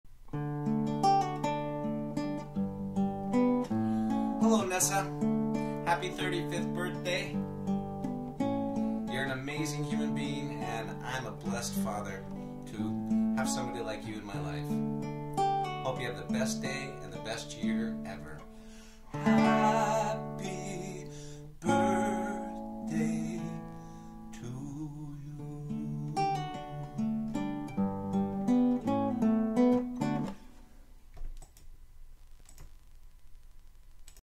Janessa 35 from Dad
Matt asked for a short Birthday message...here it is for my daughter, Janessa!